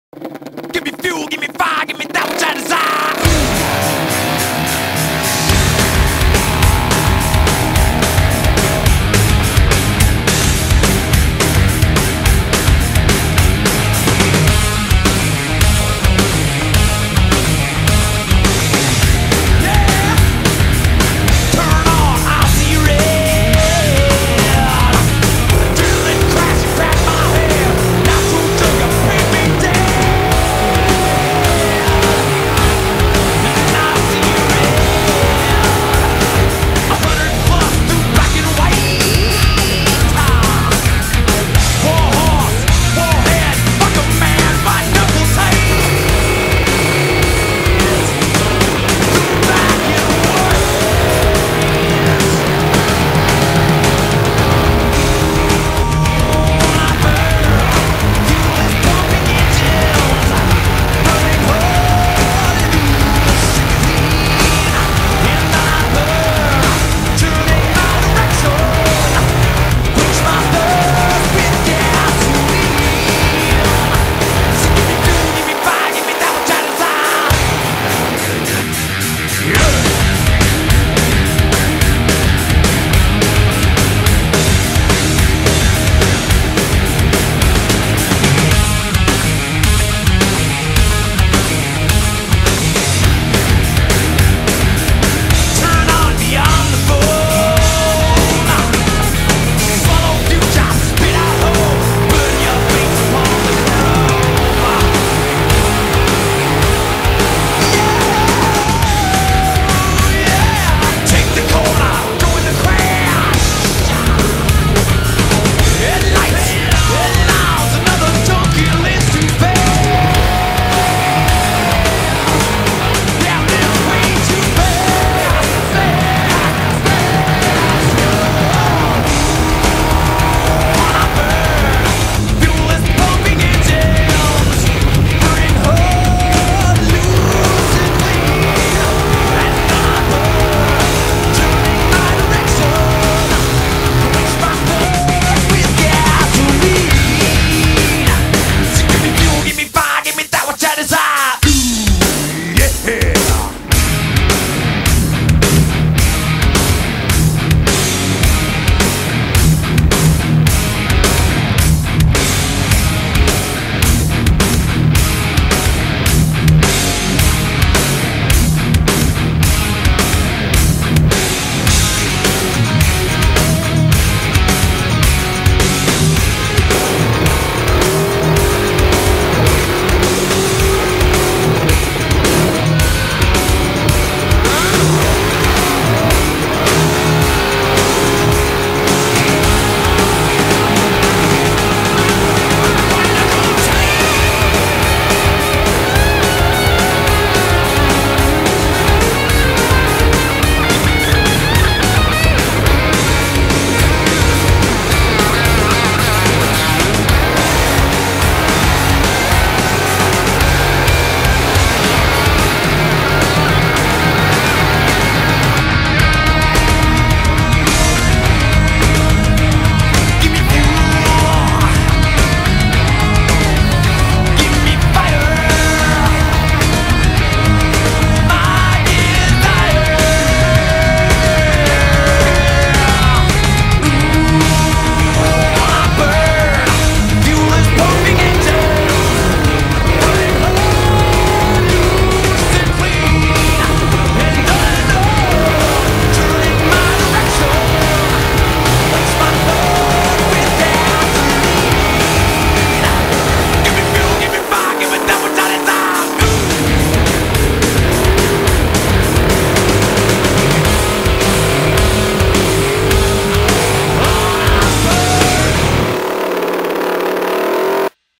Old Dumont Dunes video KFX 730 Superbeast Thanksgiving 2007
Old video of my bike at Dumont sand Dunes in 2007. The KFX 730 Superbeast